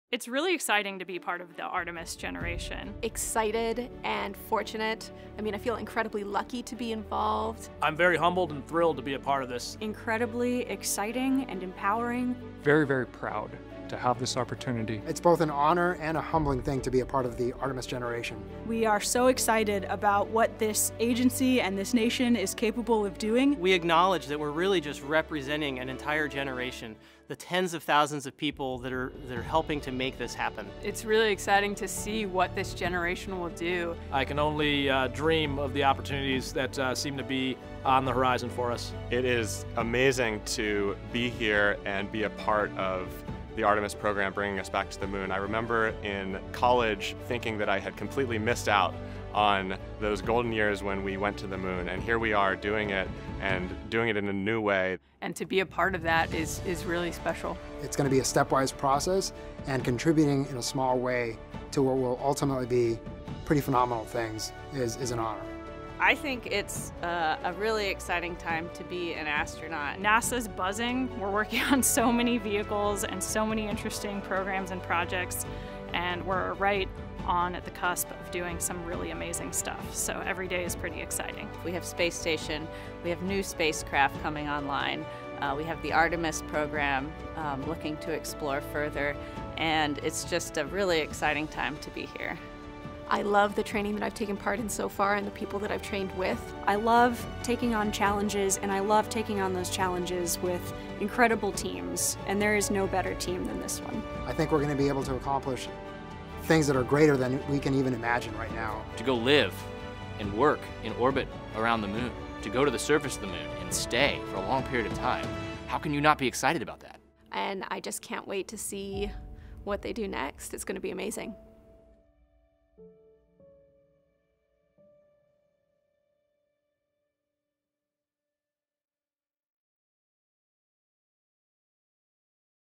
BeAnAstronaut: How Do You Feel about Being Part of the Artemis Generation?
Our newest class of astronauts will journey to the Moon, Mars and beyond. In their own words, here’s what it means to be part of the Artemis Generation. Apply to BeAnAstronaut and fly to worlds beyond your imagination